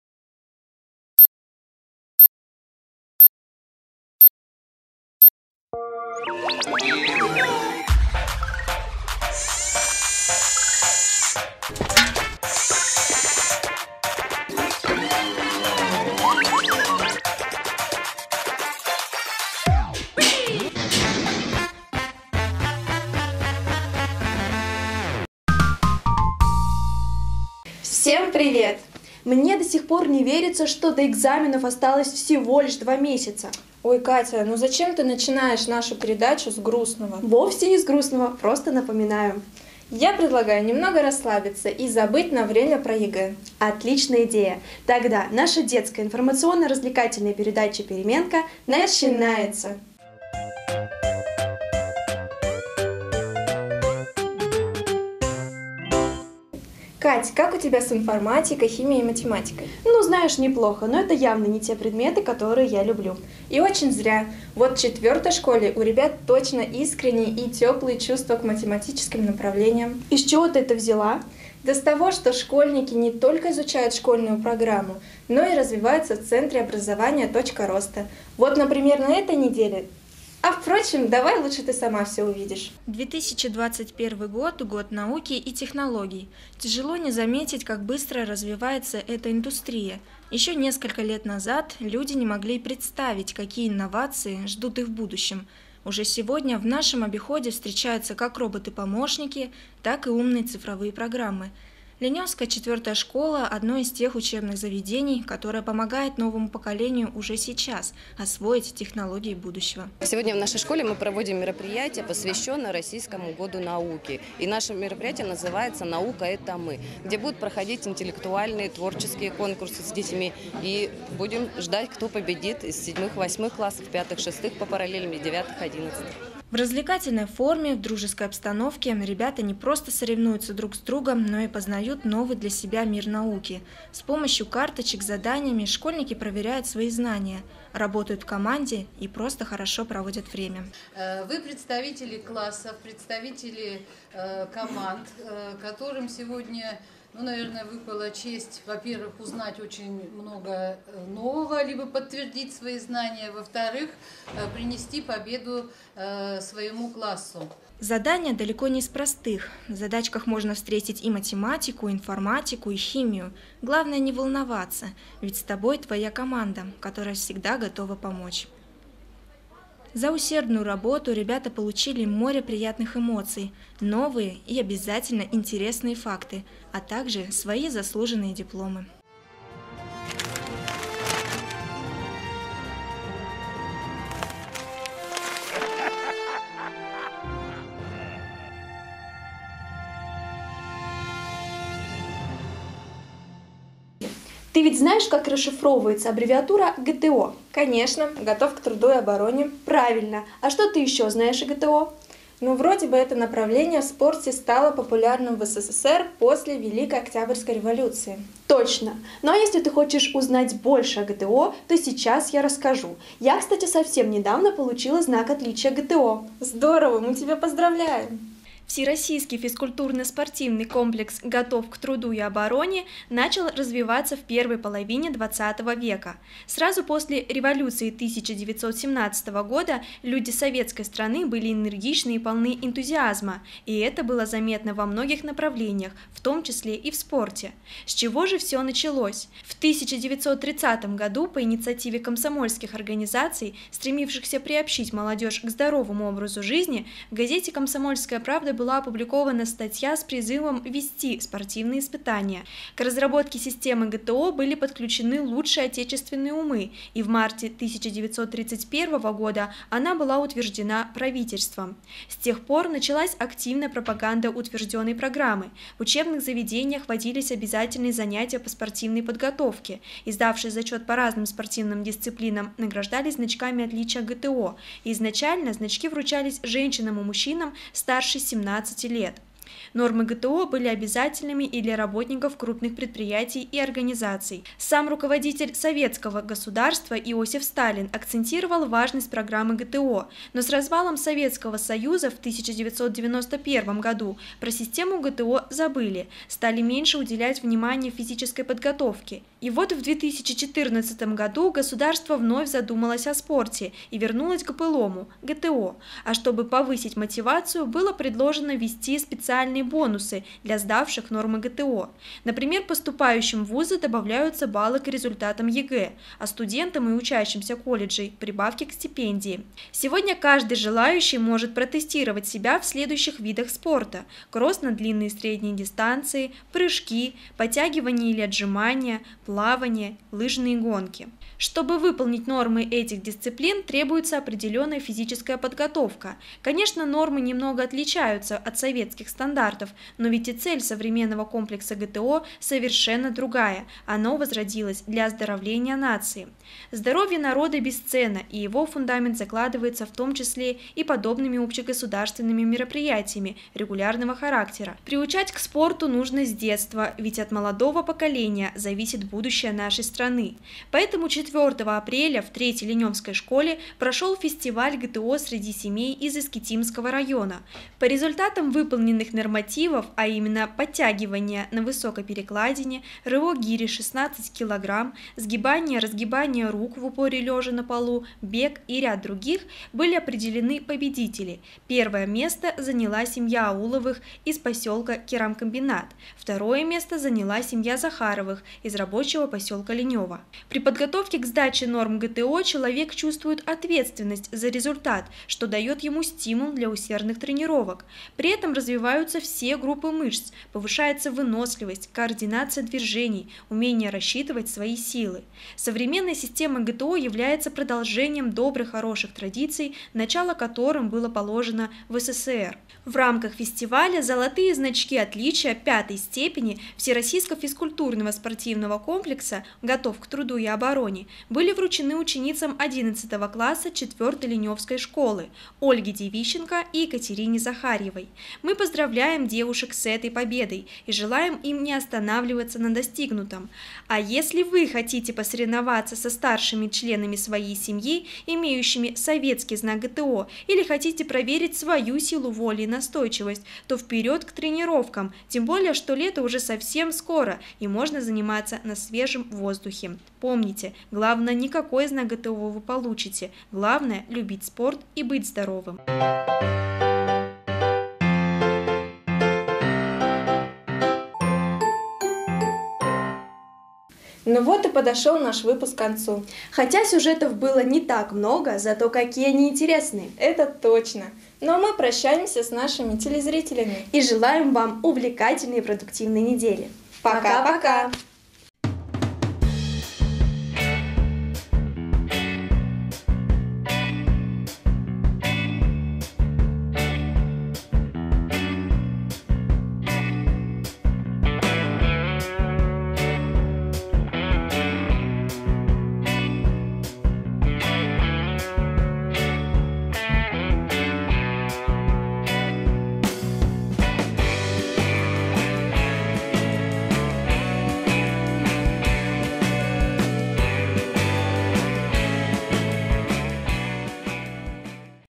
Переменка 11.04.2021
В 4й Линёвской школе прошли интллектуальные игры приуроченные ко дню науки.
А спортивные достижения линёвских школьников отмечены значками ГТО.